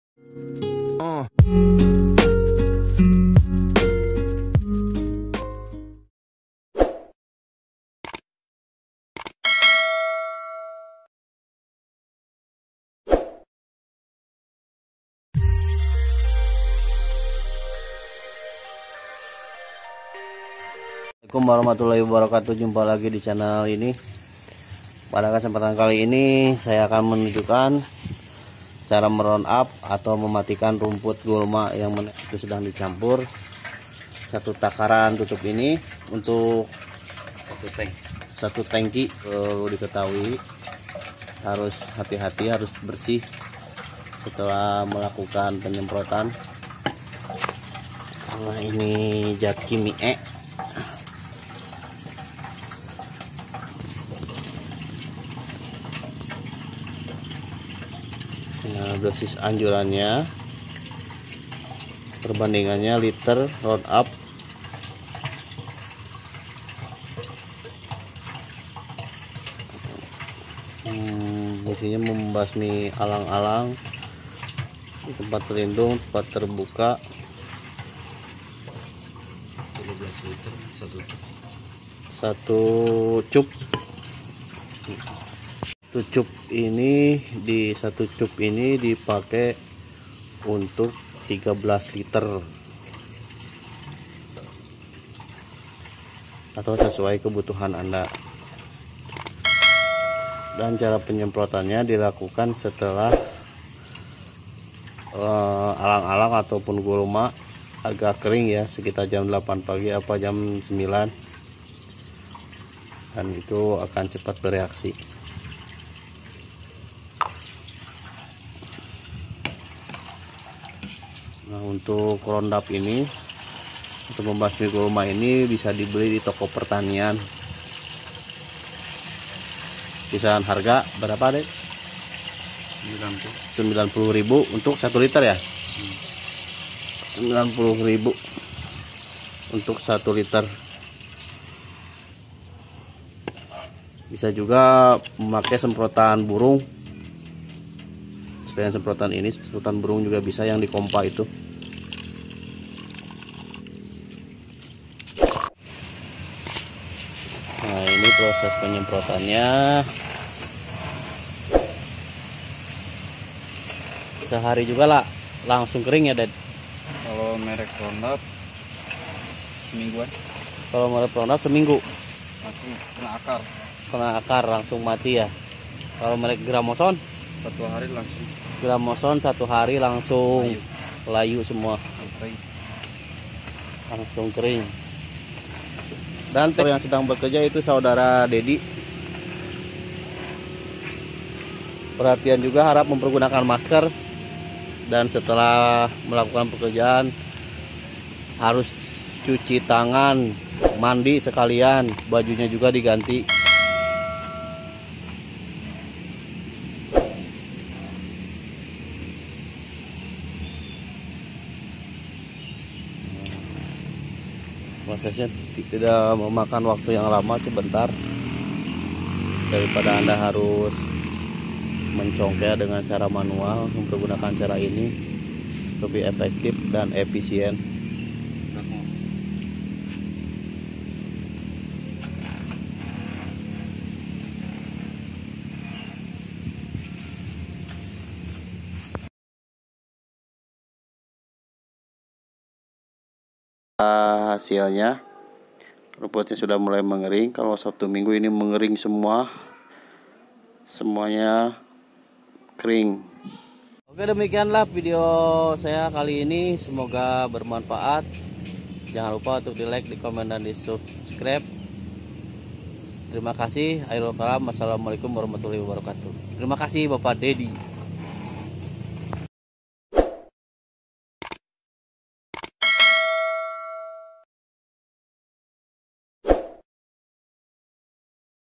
cara mematikan gulma dan alang2 dengan round up
membasmi rumput liar/ilalang dan gulma dengan menggunakan cairan roundup yang di campur dengan air